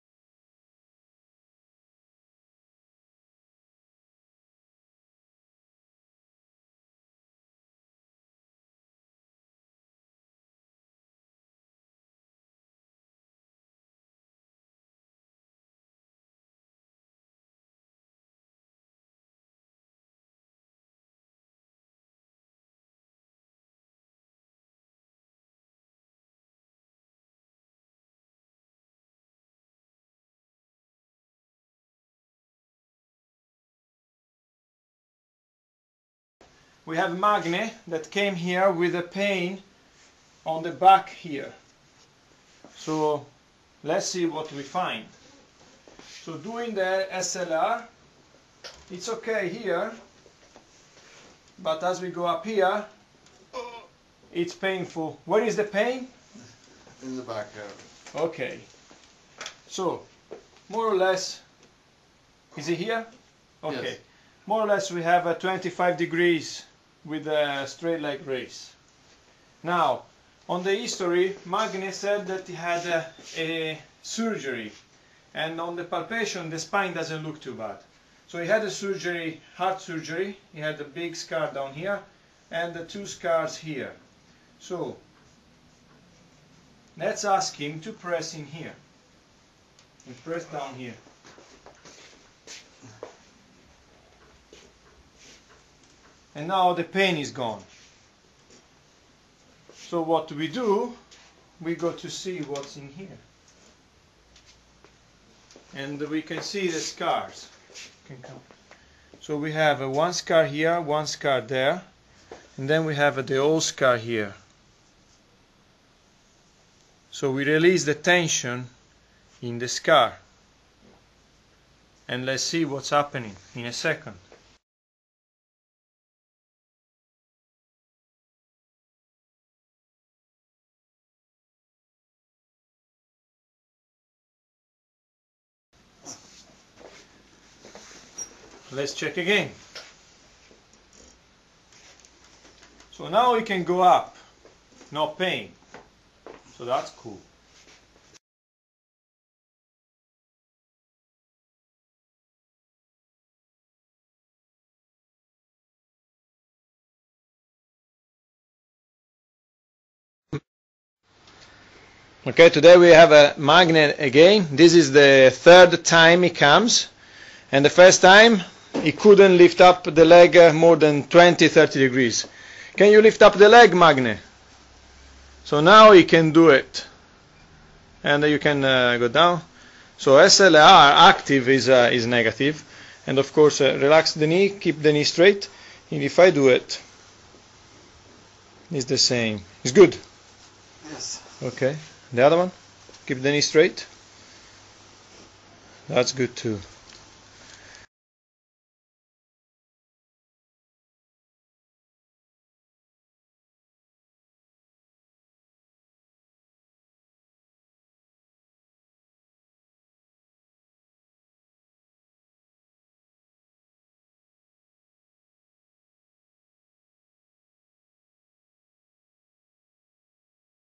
How a scar tissue can affect body functions
Here is a case of a patient that complained of right side low back pain radiating down the back of the right leg. Straight Leg Raise (SLR) orthopedic test was positive at 25-30 due to pain in the right side of the low back and on the back of the right gluteal region and thigh. The application of pressure over the scar, he has just below the sternum, markedly improved the muscles reactivity and it was possible to have a negative SLR test, throughout its range.
Anyone that has a scar in their body should found out if it is interfering with their body functions.